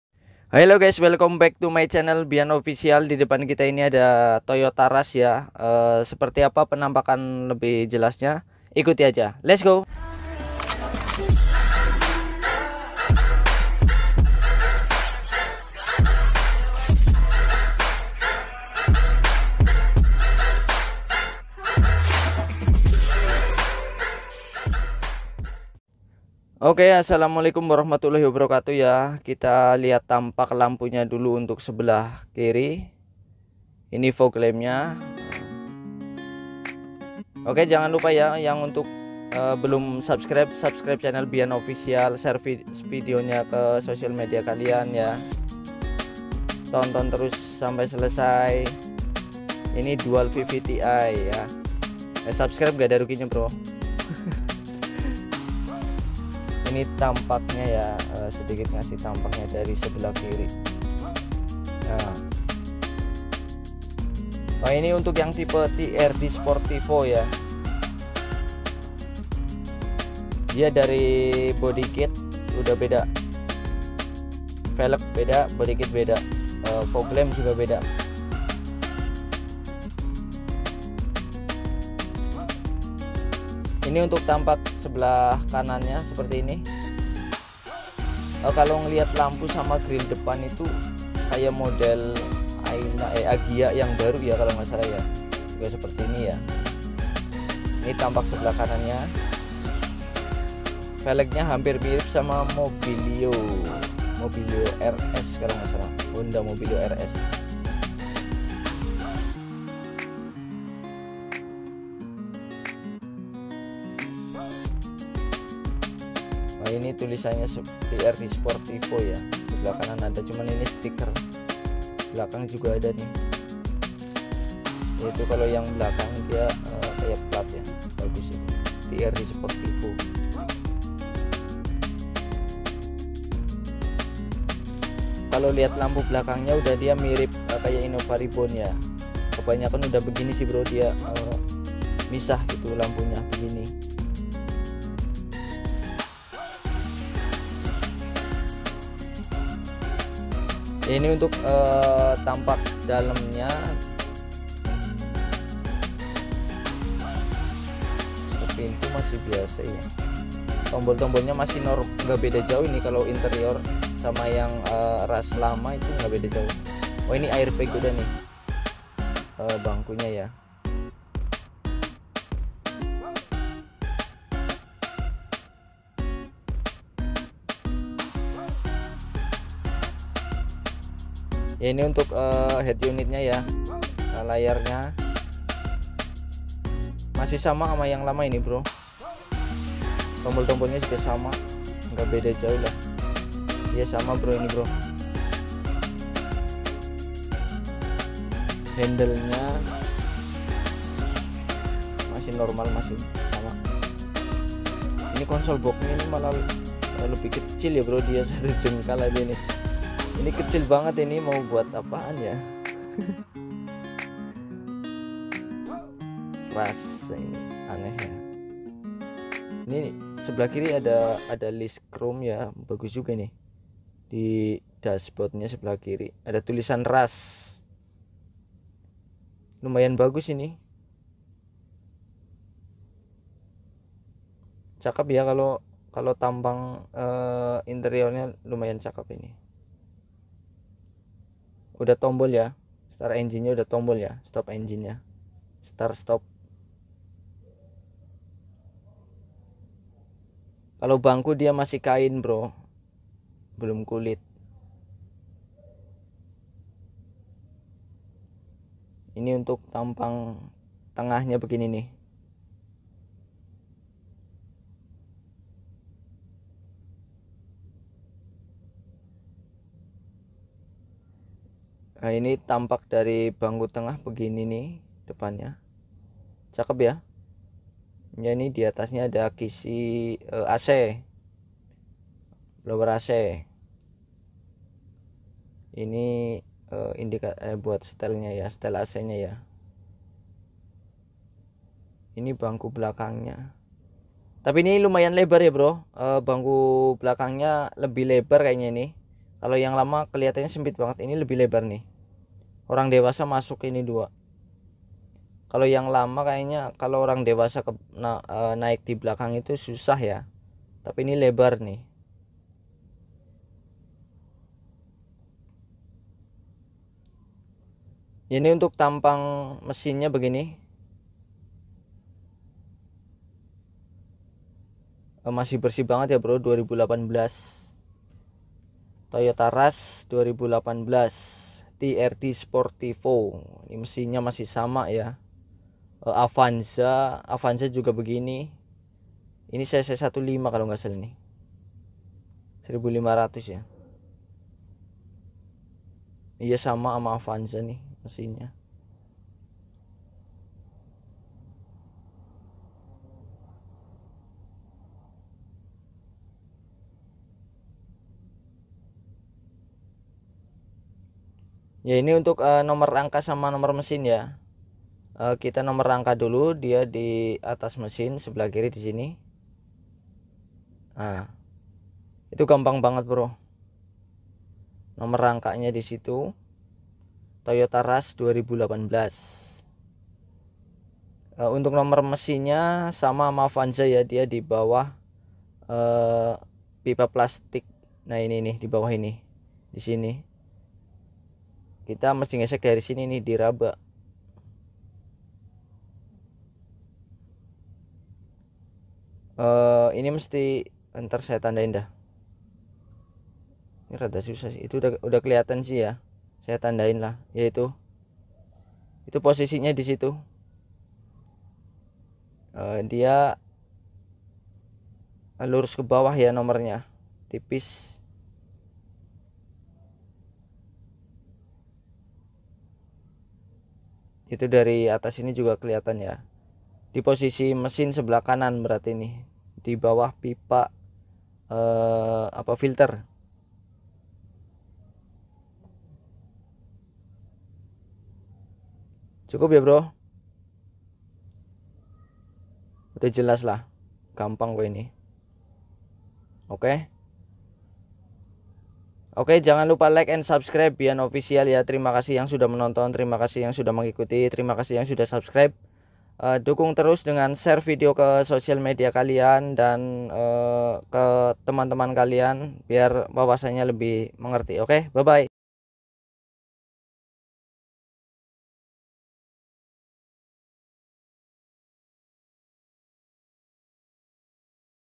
Toyota rush trd 2018 bursa mobil bekas
Toyota rush trd 2018
Review di sorum mobil seken. .
bursa mobil bekas wtc mangga dua toyota rush tipe trd untuk taunya 2018